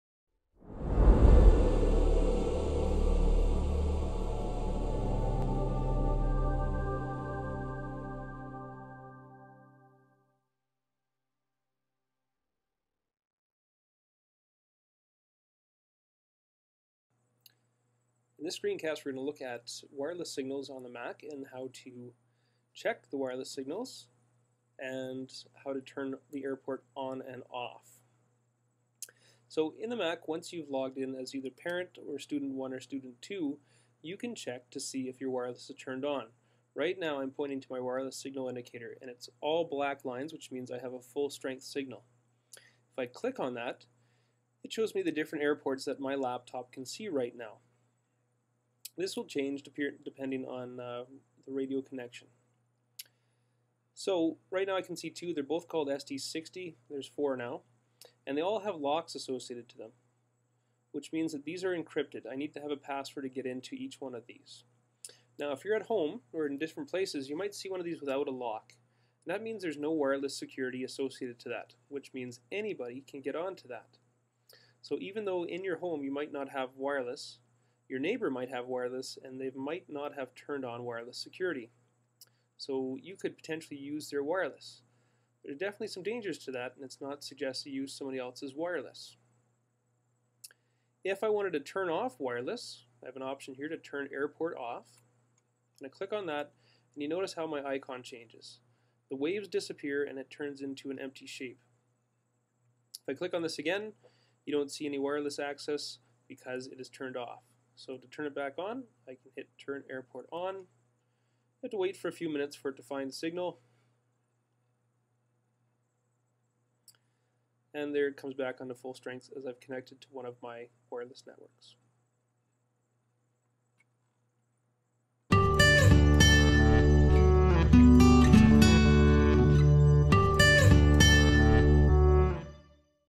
How to check the wireless signal and turn off the airport in Mac OS X
Often asked question from parents of students in our wireless writing program